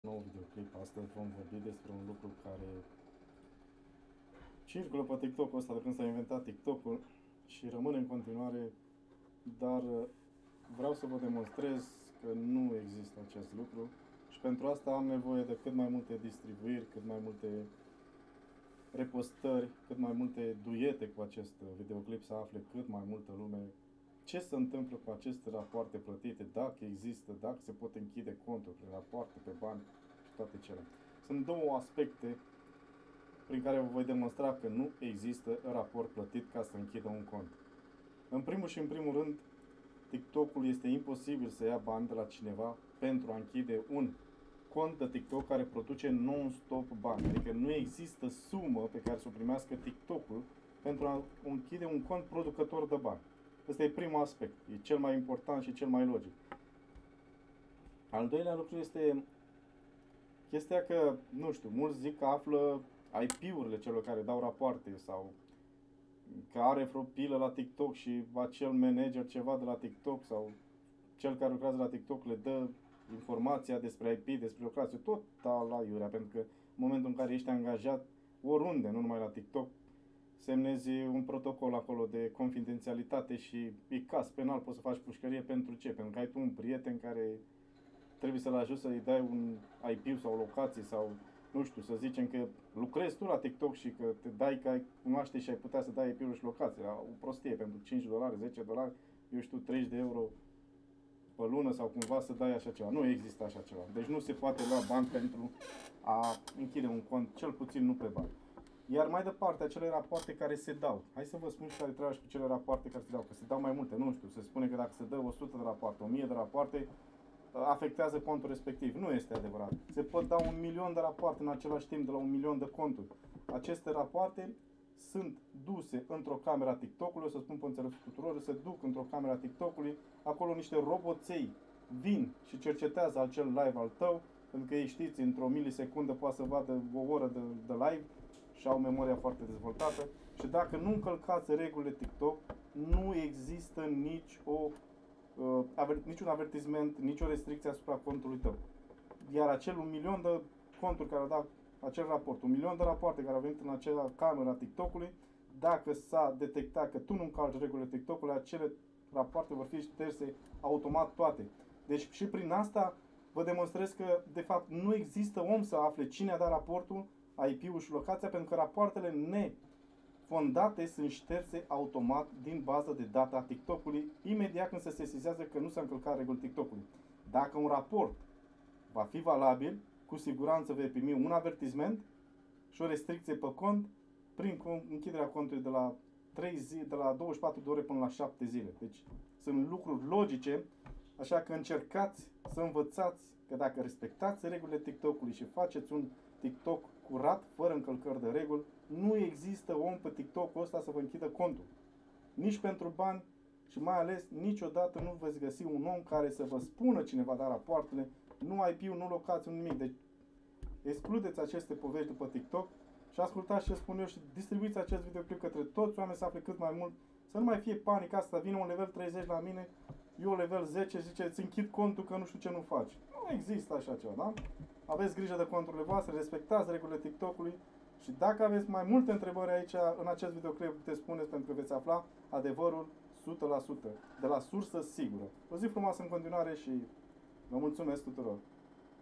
Cum Funcționează Rapoartele Plătite pe TikTok | Ghid Complet
În acest videoclip, îți explic cum funcționează rapoartele plătite pe TikTok